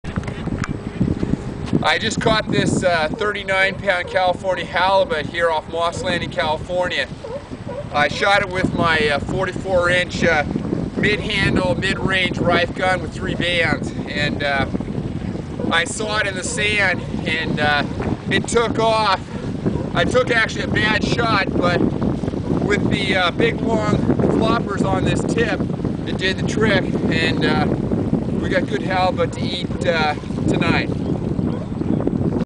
Using My RIffe Speargun
I landed this 39 lb. California halibut using my Riffe mid-handle, mid-range speargun in Monterey Bay, California.  What a thrill!